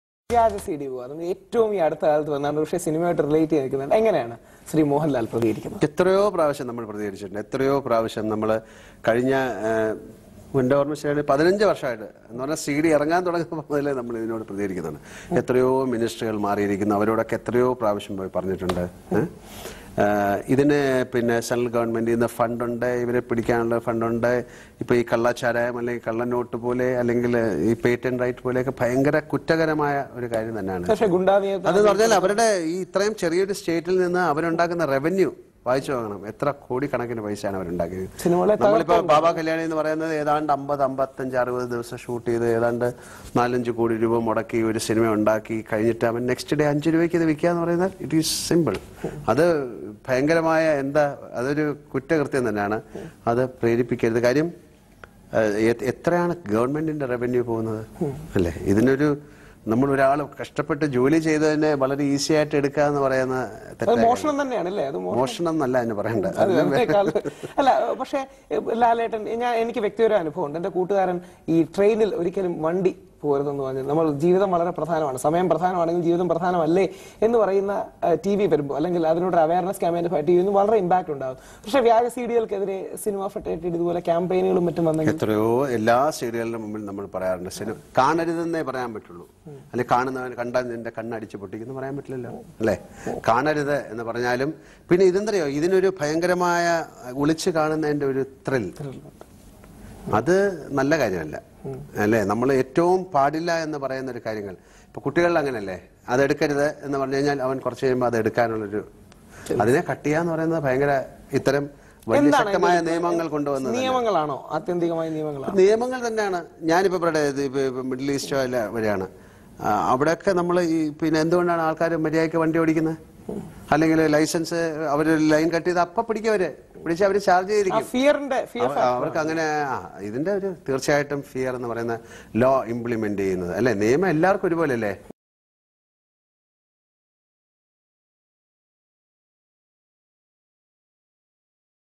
Mohanlal About Piracy
Mohanlal Talking about video piracy